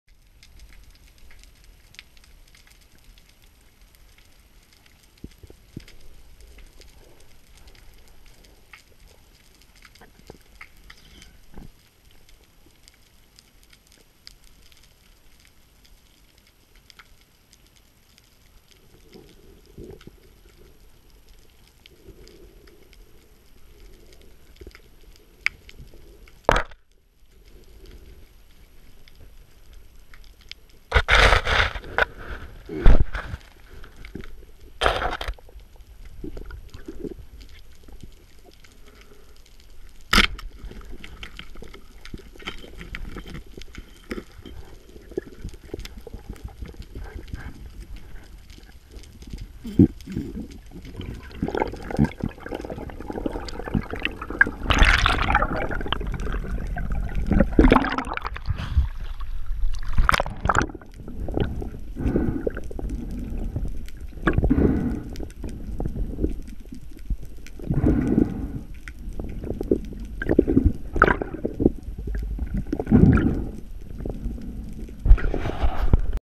Elusive Hogfish, boquinete escondido
Hogfish hidden in a small cave.
Boquinete escondido en laja...